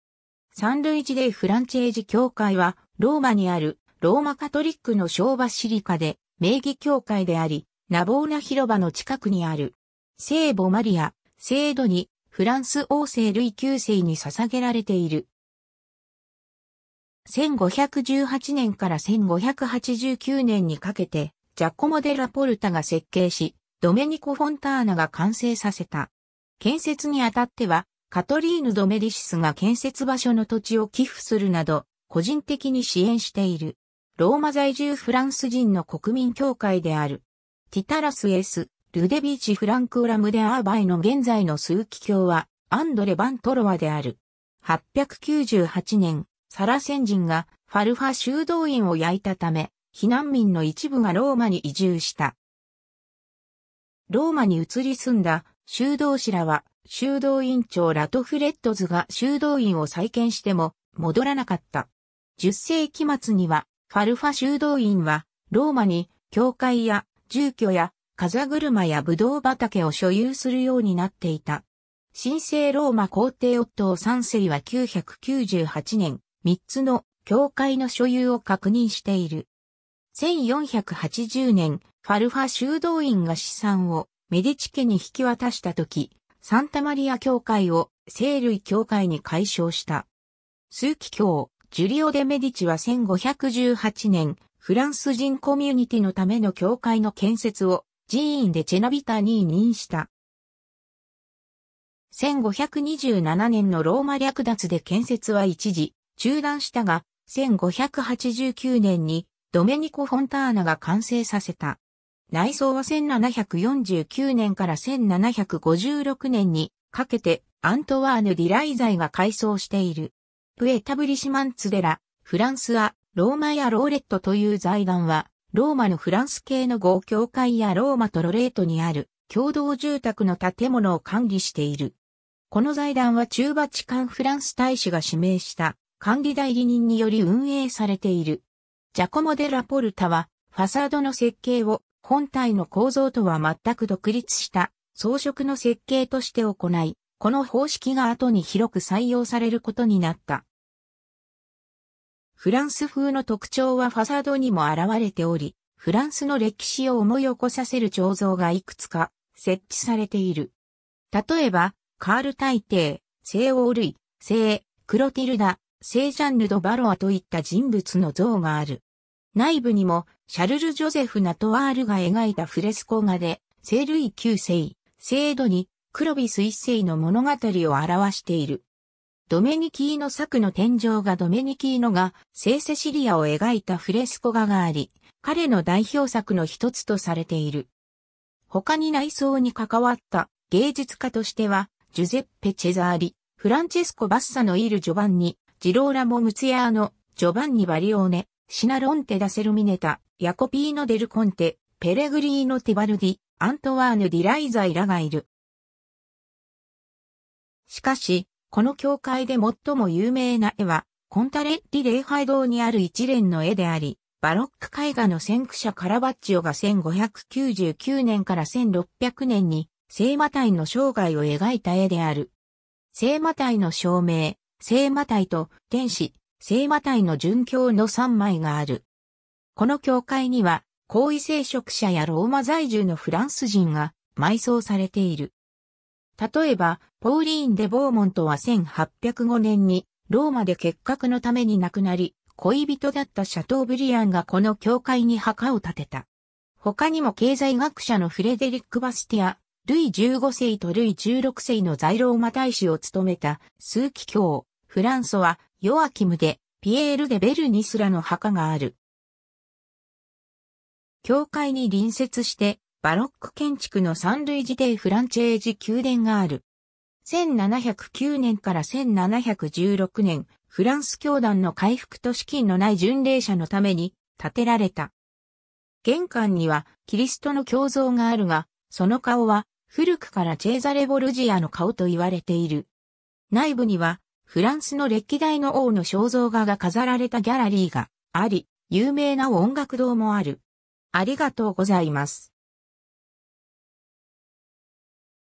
サン・ルイジ・デイ・フランチェージ教会
サン・ルイジ・デイ・フランチェージ教会（イタリア語: San Luigi dei Francesi）はローマにあるローマカトリックの小バシリカで名義教会であり、ナヴォーナ広場の近くにある。
聖母マリア、聖ドニ、フランス王聖ルイ9世に捧げられている。
1518年から1589年にかけて、ジャコモ・デッラ・ポルタが設計しドメニコ・フォンターナが完成させた。
建設にあたってはカトリーヌ・ド・メディシスが建設場所の土地を寄付するなど、個人的に支援している。
ローマ在住フランス人の国民教会である。
Titulus S. Ludovici Francorum de Urbe の現在の枢機卿は André Vingt-Trois である。
 898年、サラセン人がファルファ修道院を焼いたため、避難民の一部がローマに移住した。
ローマに移り住んだ修道士らは修道院長 Ratfredus（934年 - 936年）が修道院を再建しても戻らなかった。
10世紀末には、ファルファ修道院はローマに教会や住居や風車やブドウ畑を所有するようになっていた。
神聖ローマ皇帝オットー3世は998年、3つの教会（サンタ・マリア、サン・ベネデット、サン・サルヴァトーレ）の所有を確認している。
1480年、ファルファ修道院が資産をメディチ家に引き渡したとき、サンタ・マリア教会を聖ルイ教会に改称した。
枢機卿ジュリオ・デ・メディチは1518年、フランス人コミュニティのための教会の建設を Jean de Chenevière に委任した。
1527年のローマ略奪で建設は一時中断したが、1589年にドメニコ・フォンターナが完成させた。
内装は1749年から1756年にかけて Antoine Dérizet が改装している。
 Pieux Etablissements de la France à Rome et à Lorette という財団はローマのフランス系の5教会やローマとロレートにある共同住宅の建物を管理している。
この財団は駐バチカン・フランス大使が指名した管理代理人により運営されている。
 ジャコモ・デッラ・ポルタはファサードの設計を本体の構造とは全く独立した装飾の設計として行い、この方式が後に広く採用されることになった。
フランス風の特徴はファサードにも表れており、フランスの歴史を思い起こさせる彫像がいくつか設置されている。
例えば、カール大帝、聖王ルイ、聖クロティルダ、聖ジャンヌ・ド・ヴァロワといった人物の像がある。
内部にも シャルル＝ジョゼフ・ナトワール が描いたフレスコ画で聖ルイ9世、聖ドニ、クロヴィス1世の物語を表している。
 ドメニキーノ作の天井画 ドメニキーノが聖セシリアを描いたフレスコ画があり、彼の代表作の1つとされている。
他に内装に関わった芸術家としては、ジュゼッペ・チェザーリ、Francesco Bassano il Giovanni、ジローラモ・ムツィアーノ、ジョヴァンニ・バリオーネ、Siciolante da Sermoneta、ヤコピーノ・デル・コンテ、ペレグリーノ・ティバルディ、Antoine Derizet らがいる。
 しかし、この教会で最も有名な絵はコンタレッリ礼拝堂にある一連の絵であり、バロック絵画の先駆者カラヴァッジオが1599年から1600年に聖マタイの生涯を描いた絵である。
「聖マタイの召命」、「聖マタイと天使」、「聖マタイの殉教」の3枚がある。
 この教会には高位聖職者やローマ在住のフランス人が埋葬されている。
例えば、Pauline de Beaumont は1805年にローマで結核のために亡くなり、恋人だったシャトーブリアンがこの教会に墓を立てた。
他にも経済学者のフレデリック・バスティア、ルイ15世とルイ16世の在ローマ大使を務めた枢機卿 François-Joachim de Pierre de Bernis らの墓がある。
 教会に隣接してバロック建築のサン・ルイジ・デイ・フランチェージ宮殿がある。
1709年から1716年、「フランス教団の回復と資金のない巡礼者のために」建てられた。
玄関にはキリストの胸像があるが、その顔は古くからチェーザレ・ボルジアの顔と言われている。
内部にはフランスの歴代の王の肖像画が飾られたギャラリーがあり、有名な音楽堂もある。